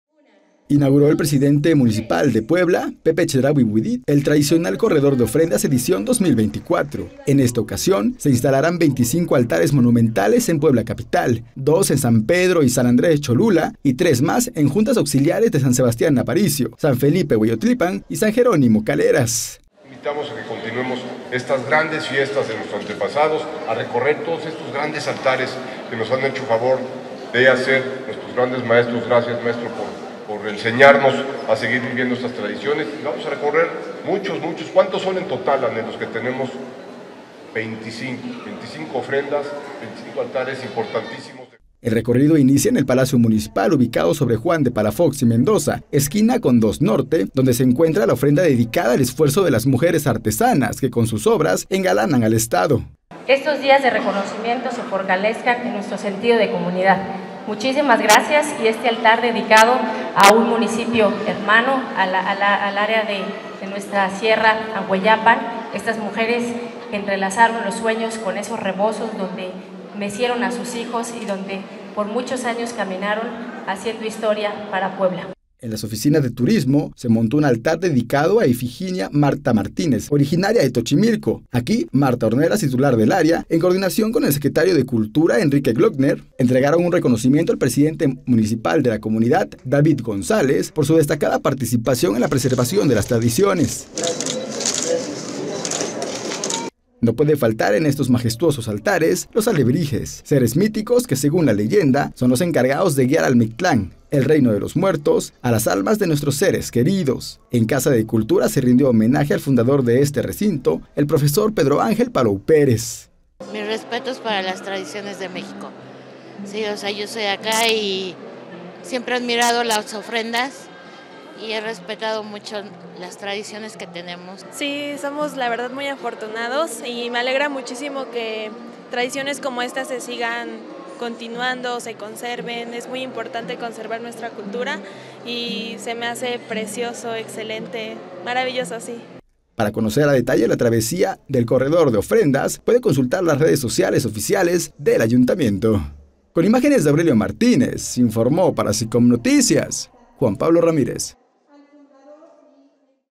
👏💀 Pepe Chedraui inauguró la edición 2024 del Corredor de Ofrendas
👏💀 Pepe Chedraui inauguró la edición 2024 del Corredor de Ofrendas; contará con 25 altares monumentales en Puebla, dos en San Pedro y San Andrés Cholula, junto con otras tres en juntas auxiliares de la capital.

Reportaje: Juan Pablo Ramírez